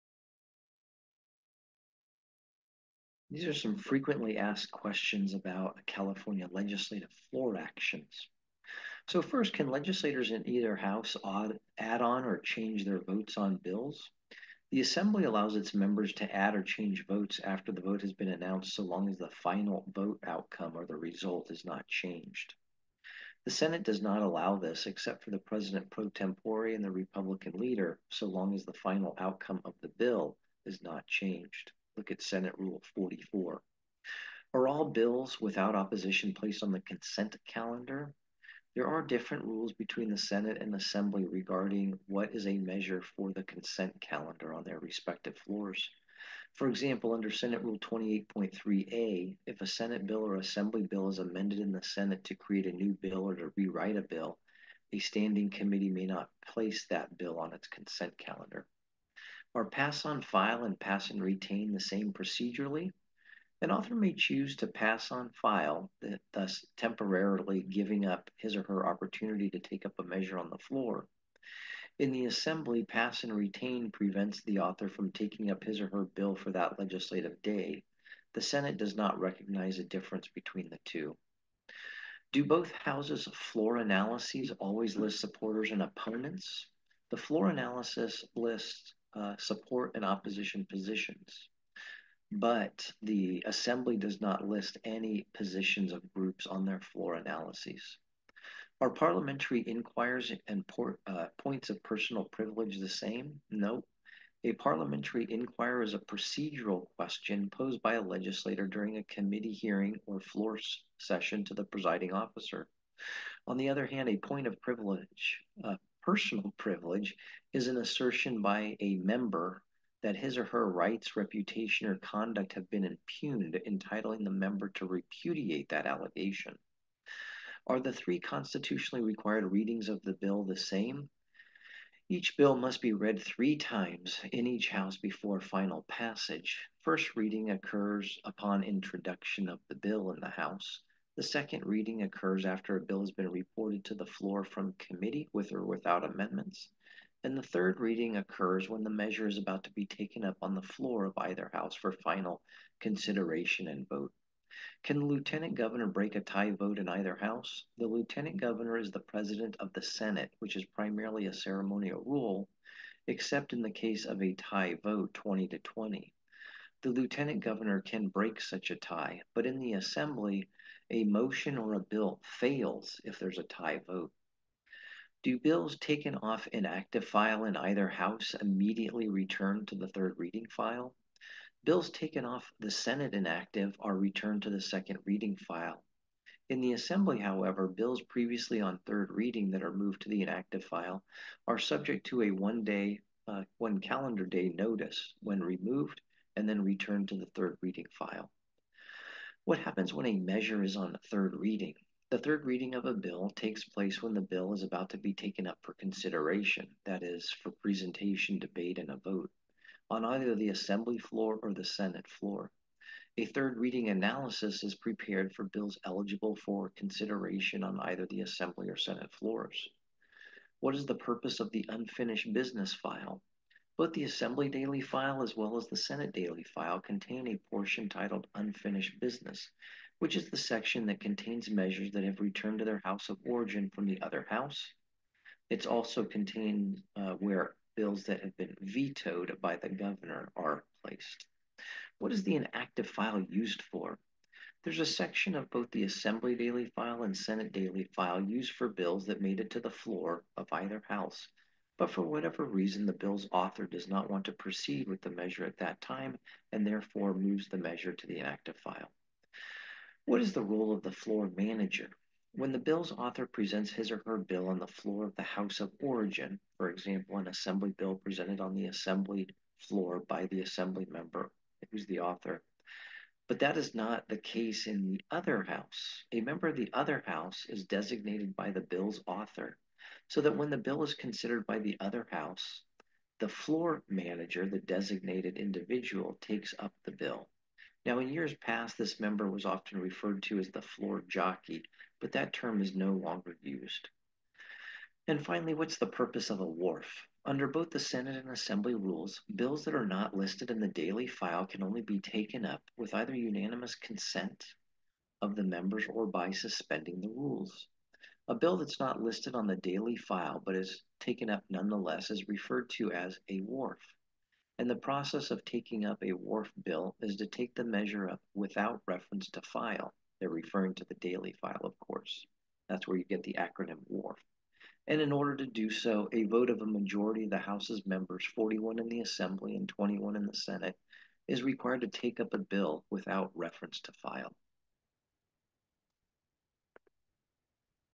FAQs on California Legislative Floor Items
This video discusses some common questions related to actions taken on the Senate and Assembly Floors in the California Legislature.